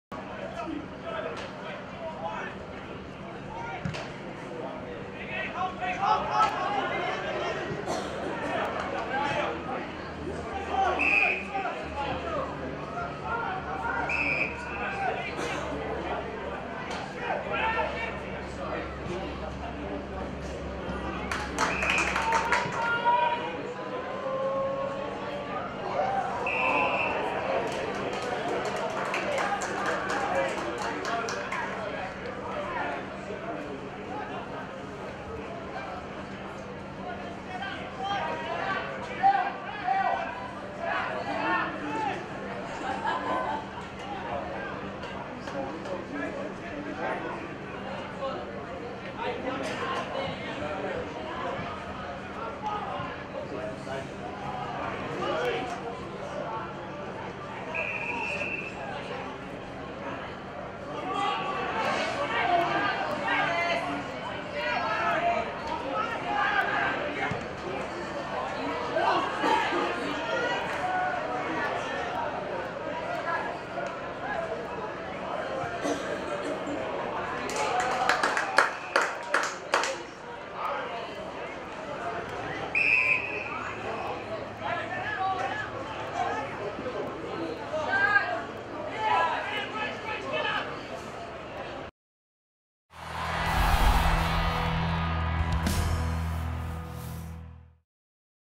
WAFL Rd 23 - Jesse Crichton
Jesse Crichton had a solid performance in the WAFL on the weekend.